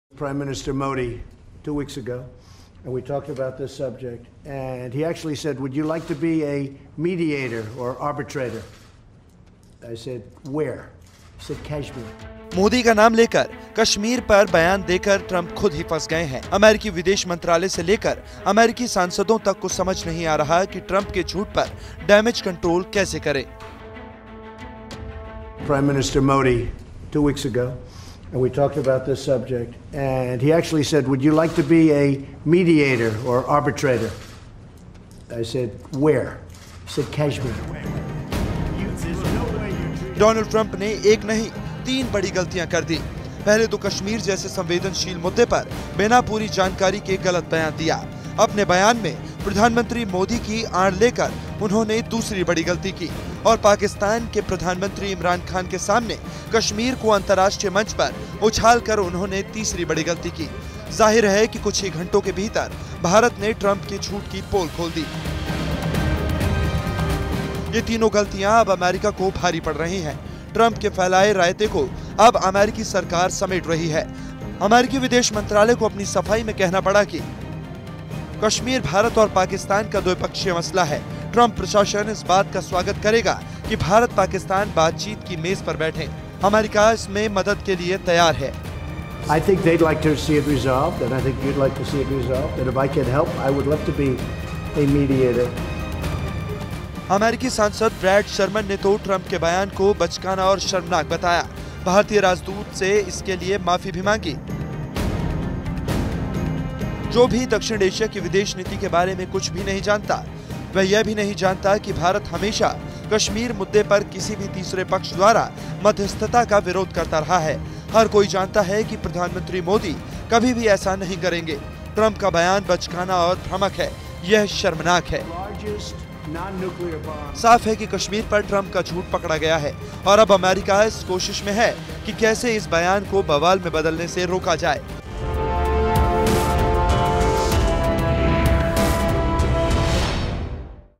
Trump के झूठ पर अमेरिका ने क्या सफाई दी? EXCLUSIVE
मोदी का नाम लेकर कश्मीर पर बयान देकर ट्रंप खुद ही फंस गए हैं... अमेरिकी विदेश मंत्रालय से लेकर अमेरिकी सांसदों तक को समझ नहीं आ रहा कि ट्रंप के झूठ पर डैमेज कंट्रोल कैसे करें ?..